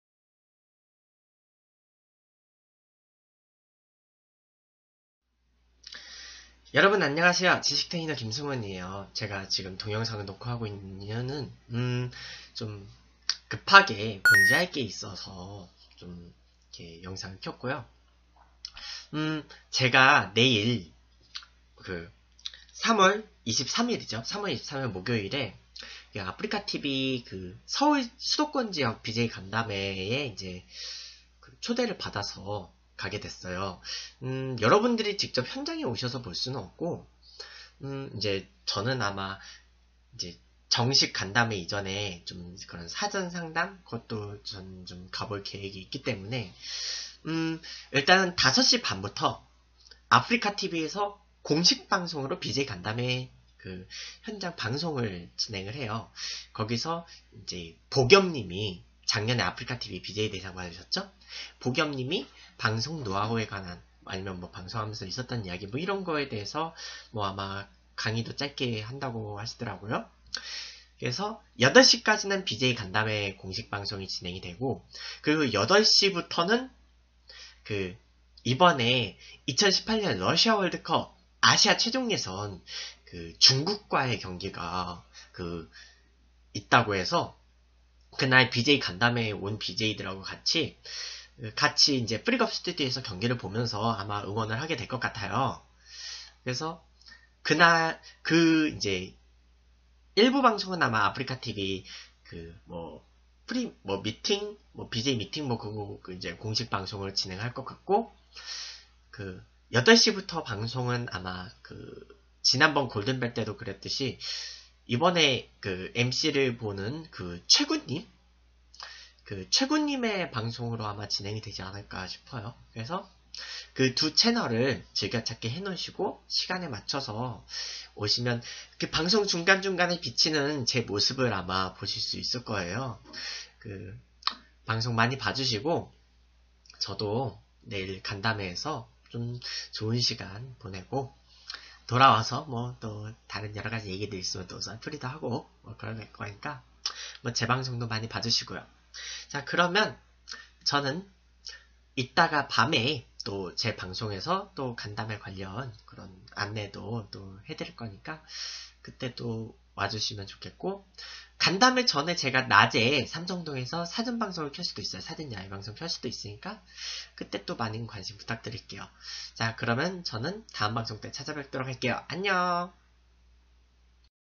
BJ간담회 및 월드컵 최종예선 공식방송 출연 | 2017.03.23 출연 아프리카TV BJ지식테이너 김승훈 Feat. 보겸, 최군, 김흥국
BJ간담회 현장 생중계
2017년 3월 23일 목요일 17:30 ~

2018 러시아 월드컵 최종예선 VS 중국
BJ간담회 종료 후 20:00~
최군 님 or 이강 님 방송에서 보세요.

지테 님의 방송국으로는 간담회 전 삼성동 사전방송이나 행사 후 뒤풀이 방송 또는 집에 도착해서 썰풀이 방송 등이 계획되어 있어요.